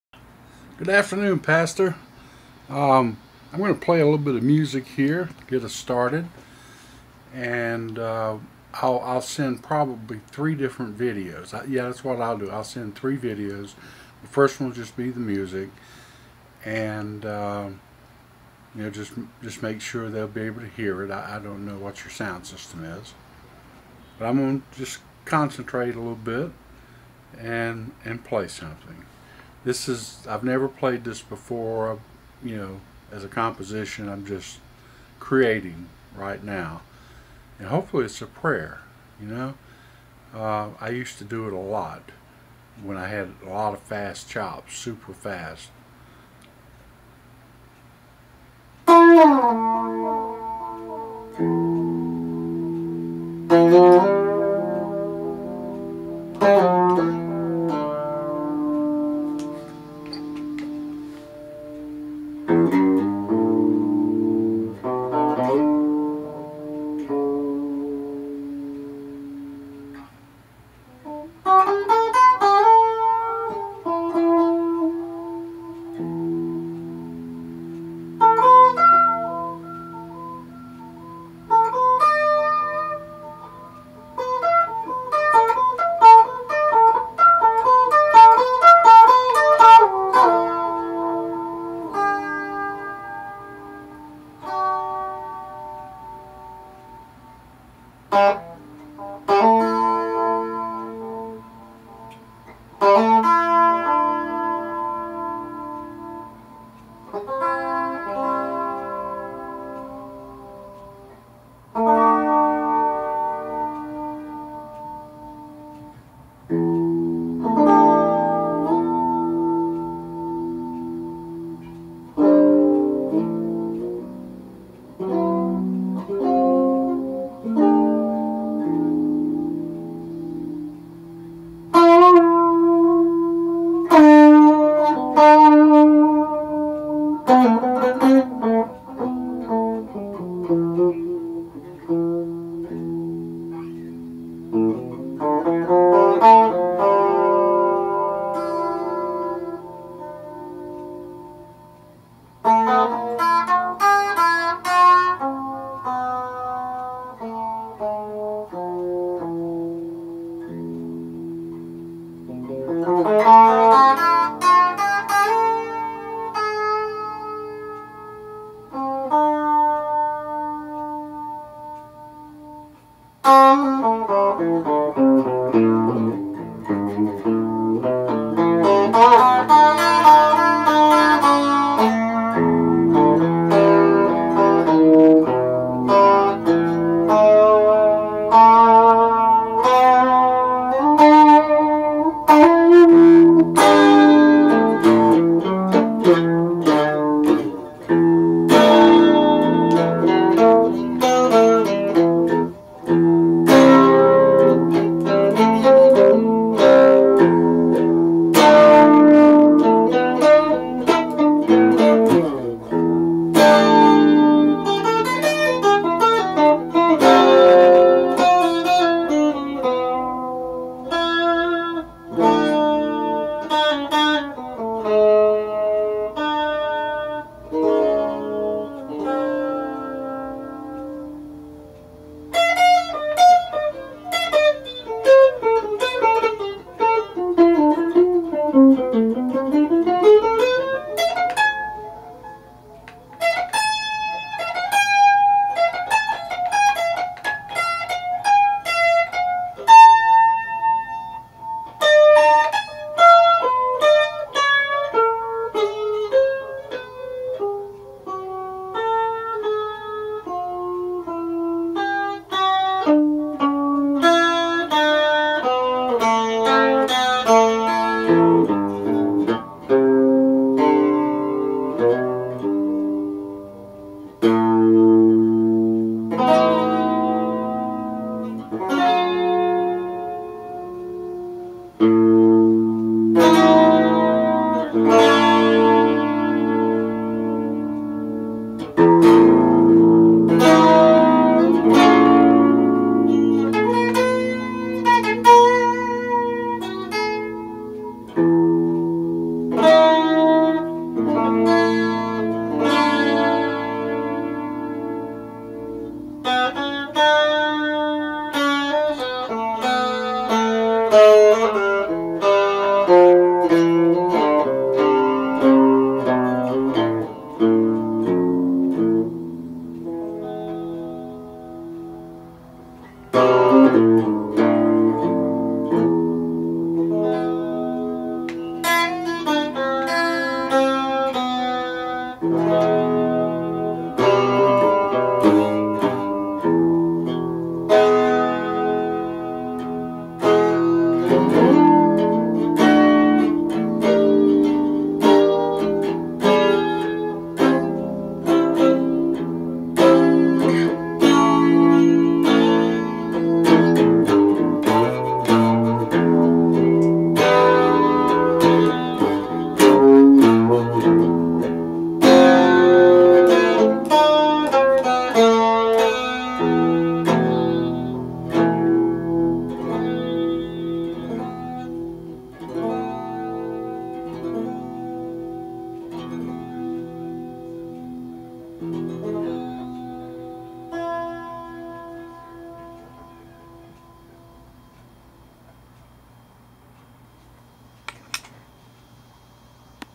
Another Take It Off The Top
Pure improv with no direction until the first note. Am feel very creative.